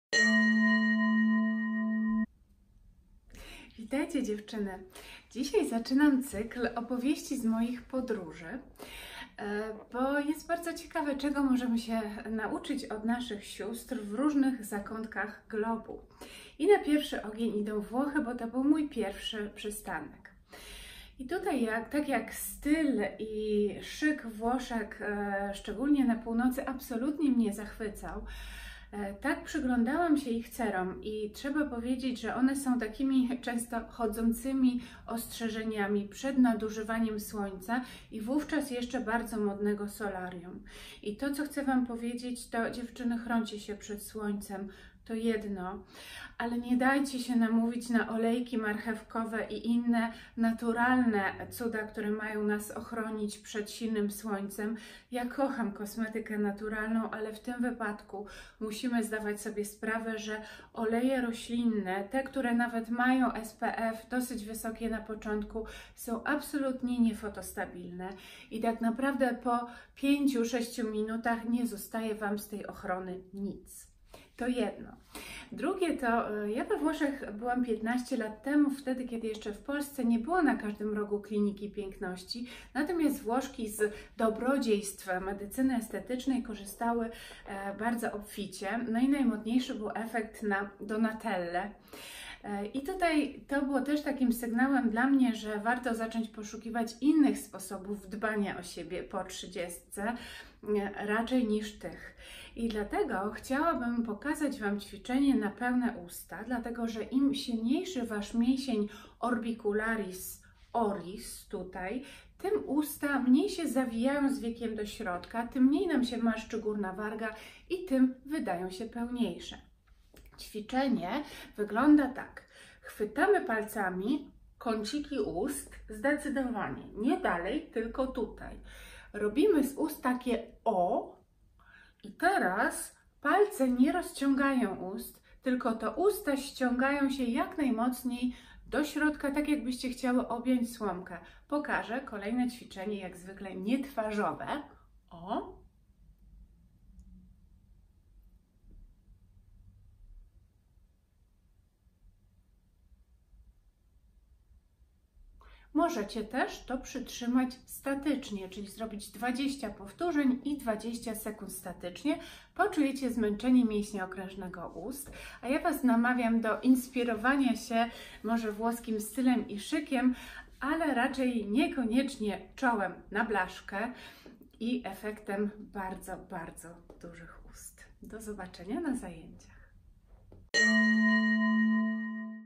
Joga Twarzy - Otwieramy cykl: Kobiece rytuały pielęgnacyjne na świecie (I jak je powtórzyć w domu)
Drogie Dziewczyny,
Zaczynam serię blogów z moich podróży i badań kobiecych trików i sposobów na urodę, z pięciu kontynentów. Czego możemy nauczyć się od naszych Sióstr w innych zakątkach globu? A czego one od nas? Jeśli macie swoje super sposoby, podzielcie się proszę nimi w komentarzach!!

Na pierwszy ogień: Włochy
Pierwszym przystankiem były północne Włochy: wspaniała architektura, rozkosze podniebienia i Włoszki: inspirujące elegancja i odwagą w doborze biżuterii, akcesoriów, zarazem jednak stanowiące chodzące ostrzeżenie przed nadużywaniem słońca i, wówczas jeszcze bardzo popularnego, solarium.
Nauczka jest prosta: patrząc na ich przedwcześnie porzeźbione bruzdami twarze można się przekonać, że nawet oliwkową cenę trzeba chronić.

Olejek z marchewki i inne cuda
Nie dajcie się namówić na stosowanie naturalnych olejków w charakterze filtrów na mocne słońce. Idea jest kusząca, musicie jednak wiedzieć, że olejki nie są fotostabilne. Po zaledwie 5-6 minutach ochrona spada o połowę, a potem już nic z niej nie zostaje.

Opalenizna od środka
Teraz jest najwyższy czas, by zacząć brać astaksantynę, 8 mg dziennie, dla przygotowania cery na lato. To nie wykluczy filtrów, ale unikniecie przebarwień. Warto, tym bardziej że na dostawie tak silnego przeciwutleniacza skorzysta całe ciało! Ta „królowa przeciwutleniaczy” sprawi, że opalenizna będzie bardziej równomierna, a skóra zachowania dłużej swoją sprężystość.

Efekt Donatelli
We Włoszech mieszkam przez trzy lata, w czasach, gdy w Polsce „poprawianie urody” jest tylko domeną aktorek i bardzo zamożnych pań. Tam natomiast jest to na porządku dziennym i to najlepiej „na Donatellę”. Wtedy też, rozważając moje opcje tuż po trzydziestce upewniam się, że efekt estetyczny inwazyjnego odmładzania mi nie odpowiada i inspiruje raczej do poszukiwań naturalnych działań dla urody, porównywalnych skutecznością lecz o dyskretnych efektach.

Ćwiczenie na pełne usta
Im silniejszy Twój Orbicularis Oris, czyli pierścień mięśniowy wokół warg, tym wargi wydają się pełniejsze i mniej wokół nich bruzd.
Jest on tez ważnym elementem całości rusztowania mięśniowego przeciwstawiającego się grawitacji. Osłabiony, obciąża całość konstrukcji ku Matce Ziemi.

Jak go ćwiczyć intensywnie?
Chwyć kąciki ust palcami wskazującymi i kciukami, zaokrąglaj wargi i zawiń je lekko do środka. Nie rozciągaj ust nadmiernie na boki!
Teraz wyobraź sobie, ze pierścień mięśniowy wokół ust ściąga palce do środka tak, jakbyś wargami chciała objąć słomkę. Palce stawiają opór. Powtórz pulsacyjne 15 razy a następnie utrzymaj maksymalne napięcie na 15 sekund. Po odpoczynku powtórz ćwiczenie.

W razie wątpliwości zapraszam Cię na zajęcia Yogattractive, gdzie ćwiczymy i masujemy twarz dla naturalnego jej ujędrnienia.

Elegantka z Emilia-Romagna
Skłaniałbym się do zapatrzenia na włoski styl i szyk w ubiorze, doborze dodatków i stawianiu na jakość akcesoriów. Raczej z większym dystansem do dobrodziejstw mega-warg i czoła „na blaszkę”.

Teraz jest jeszcze łatwiej! Możesz zarezerwować sesję przez skype w naszym sklepie internetowym na
Zapraszamy na zajęcia i warsztaty na
Wszystkie moje vlogi znajdziesz na: